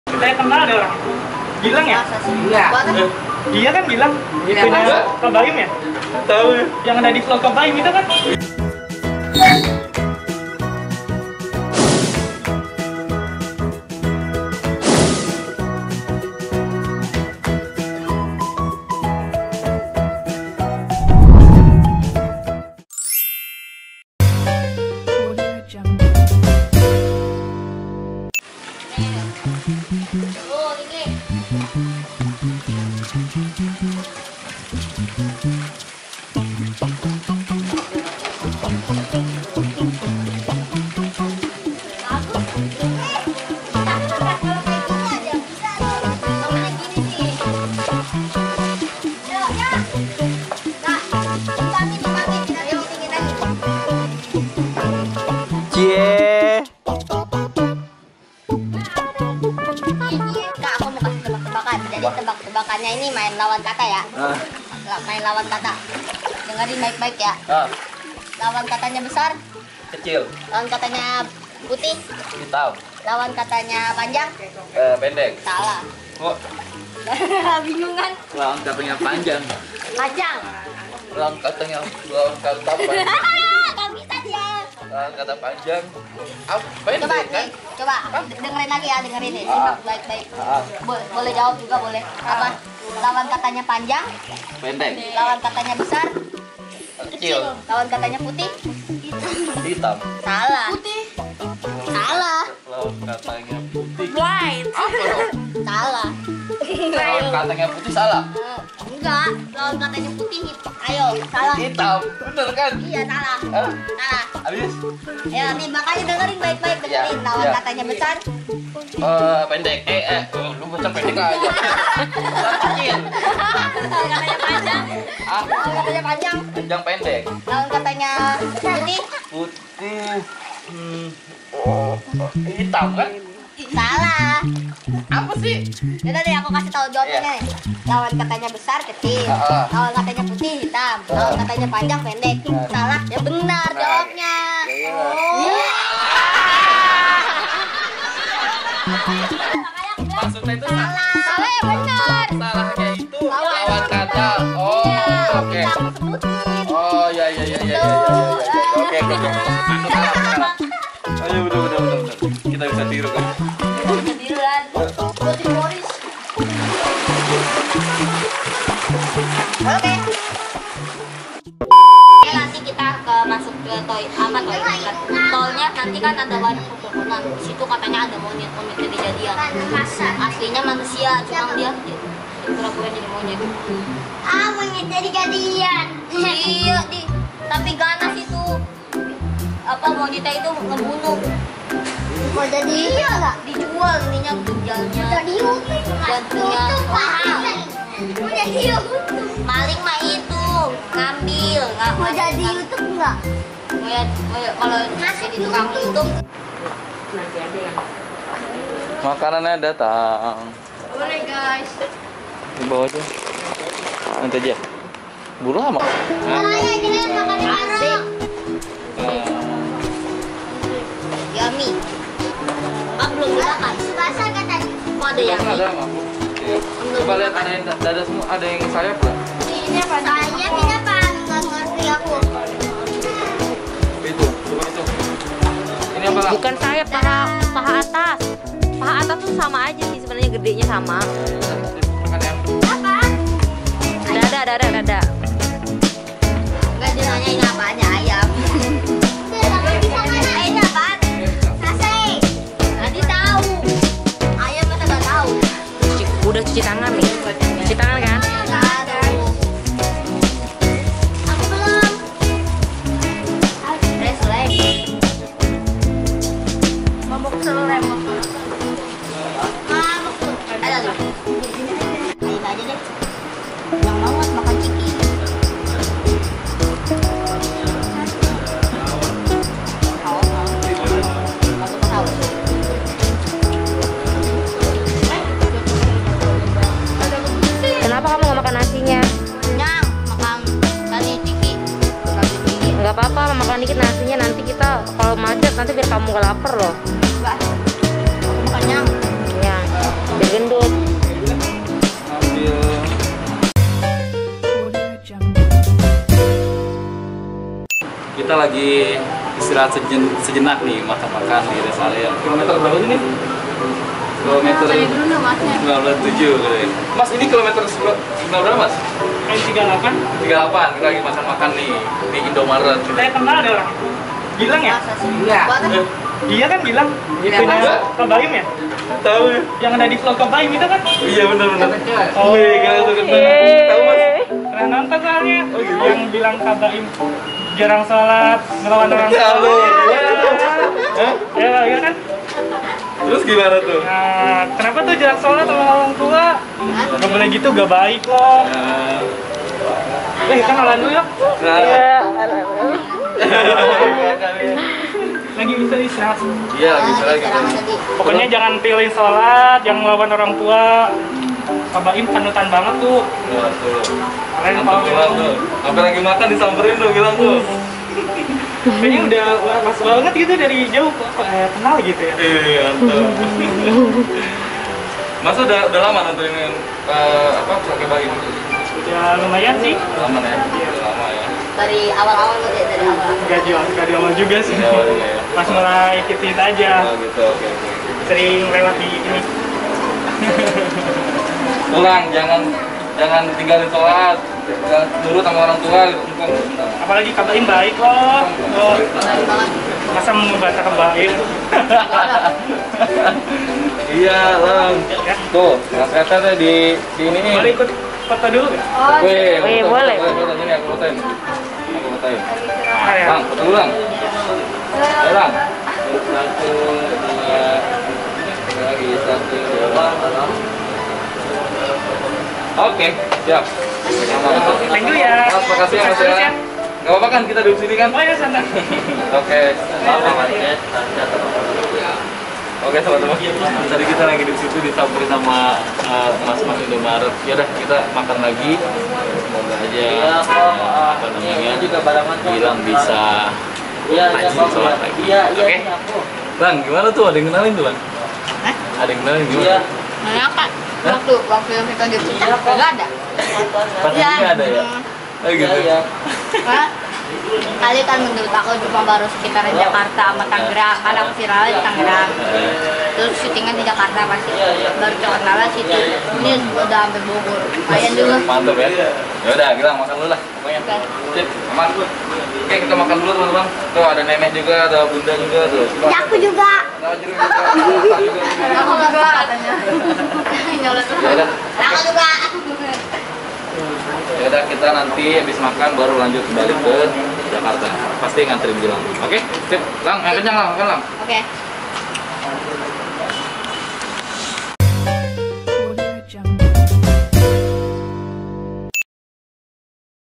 GILANG BAIM WONG DI NASEHATIN ORANG DIJALAN ! BEGINI JAWABAN GILANG ???
Semoga Gilang Ramadhan / Piano Tiger Wong sehat terus !

Semoga Teman - Teman tersentuh dengan ada nya Video ini dan bisa Memotivasi Teman - Teman semua yg Nonton Video ini dan bisa lebih peduli lagi antar sesama manusia :)